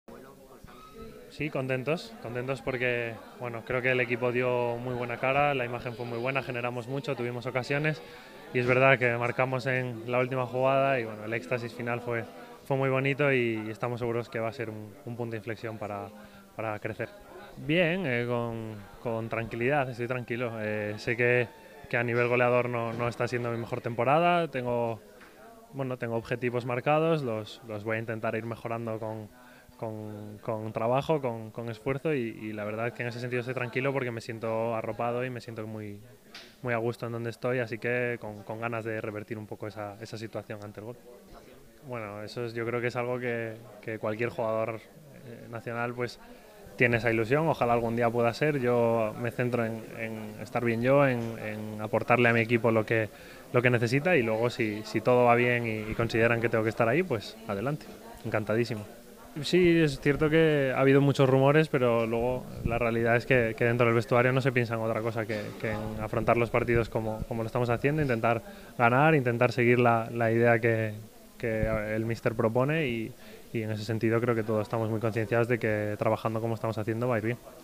Borja Iglesias: "Espero revertir mi situación ante el gol" | Diario AS
El jugador del Betis comentó en un acto de Fundación AFE y Ecoembes, que espera revertir su situación goleadora.